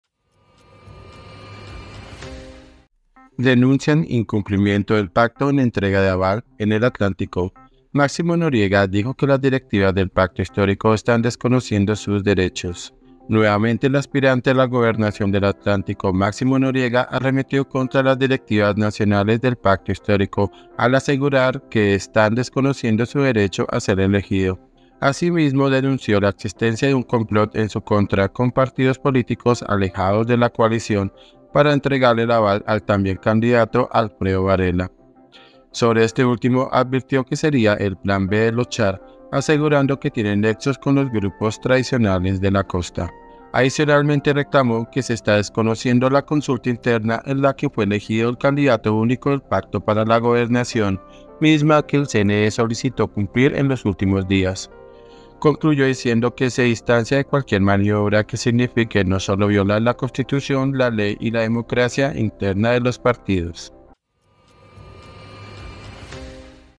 🛑 DENUNCIAN INCLUMPLIMIENTO DEL PACTO HISTÓRICO EN ATLANTICO
Nuevamente, el aspirante a la Gobernación del Atlántico, Máximo Noriega, arremetió contra las directivas nacionales del Pacto Histórico, al asegurar que están desconociendo su derecho a ser elegido.